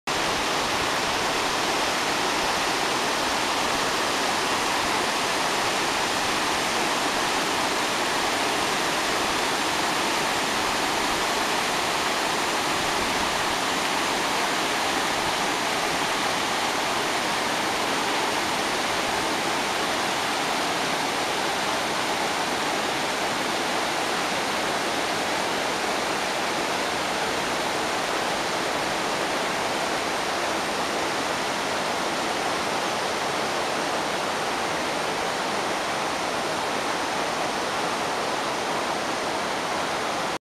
Assut de Cullera tras la gota fría, 1 de octubre (1/3)
Dos días después de finalizar las lluvias, el río vuelve a su cauce habitual, no sin haber dejado secuelas patentes en la vegetación ribereña. Aunque las aguas siguen bajando turbias y l'assut de Cullera (Situado entre los términos municipales de Sueca y Fortaleny) todavía desagua con fuerza, a lo largo del invierno viniente las riberas del Júcar volverán a lucir como si nada hubiese ocurrido, salvo que vuelva a producirse otro episodio de lluvias abundantes.